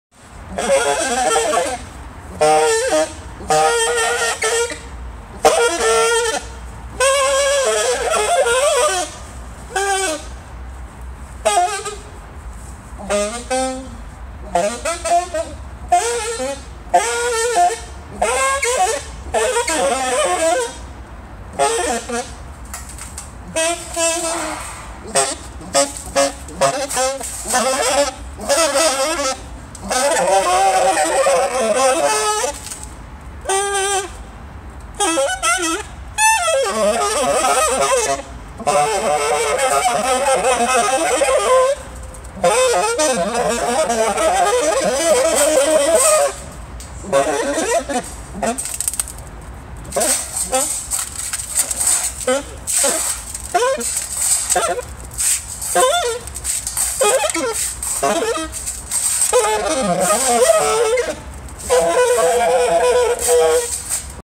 Maoist Mojo Playing in Fujin Park
After we got kicked out of Space 631 we went to the park so Maoist Mojo could play his set. We also got kicked out of the park.